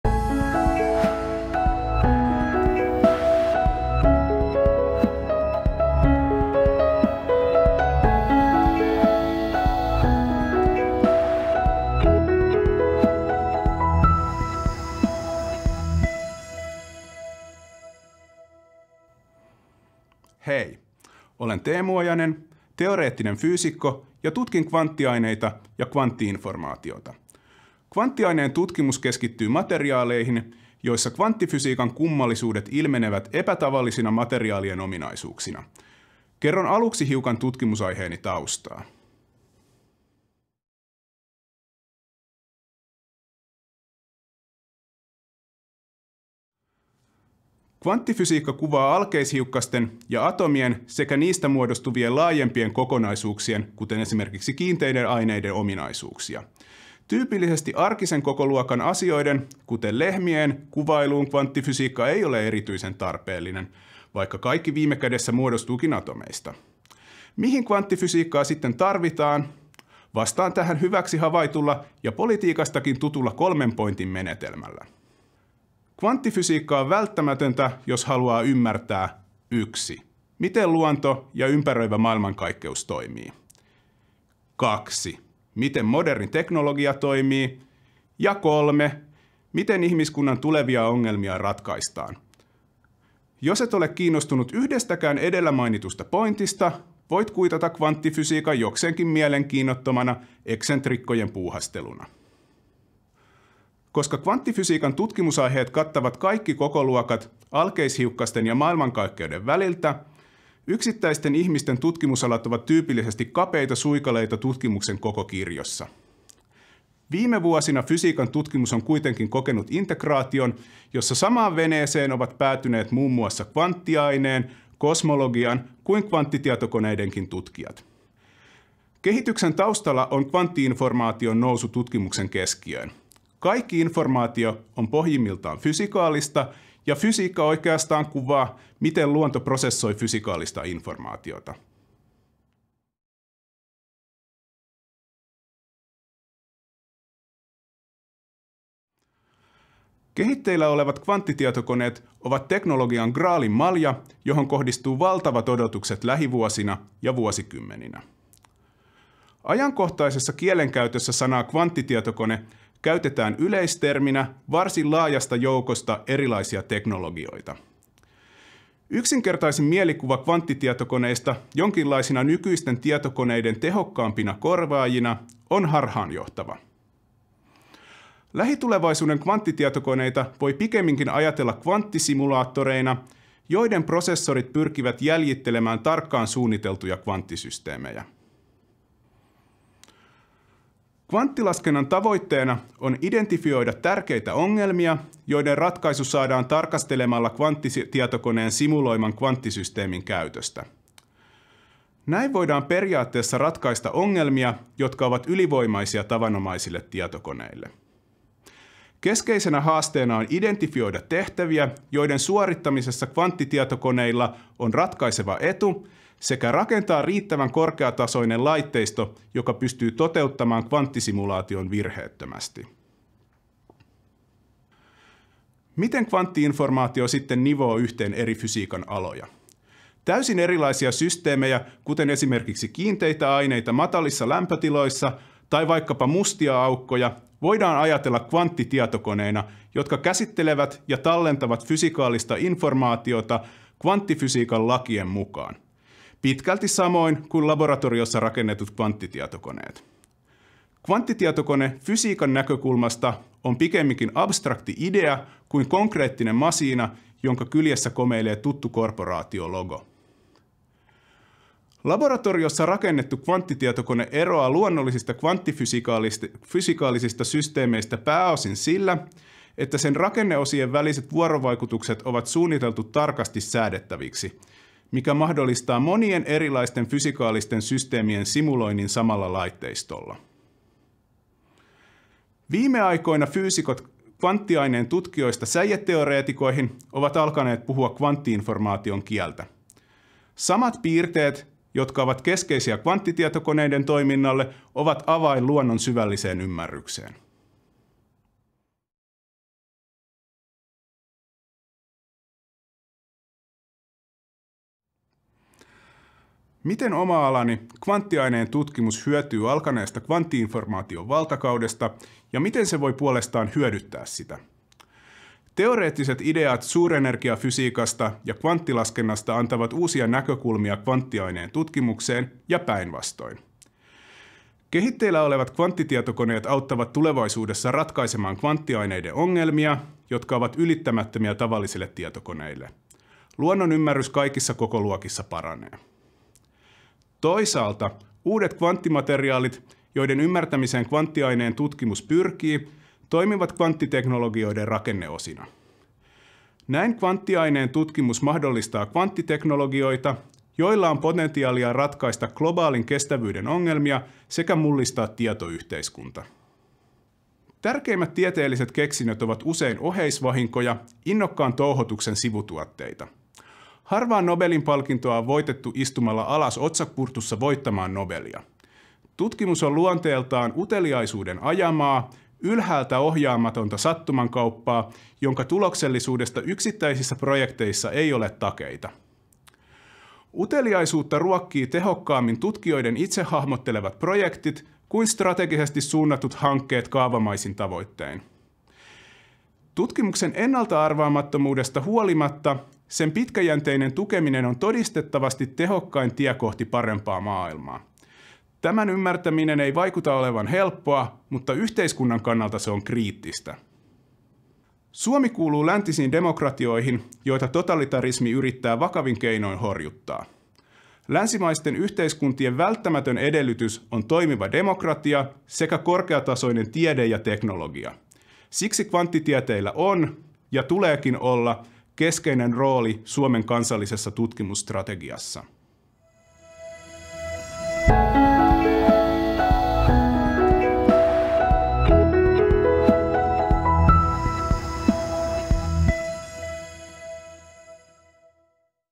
Teemu Ojanen: Kvanttitulevaisuus
Teemu Ojanen on laskennallisen fysiikan professori tekniikan ja luonnontieteiden tiedekunnassa. Hänen alaansa on kvanttiaineen tutkimus, joka keskittyy materiaaleihin, joissa kvanttifysiikan kummallisuudet ilmenevät epätavallisina materiaalien ominaisuuksina.

Subtitles in English and in Finnish.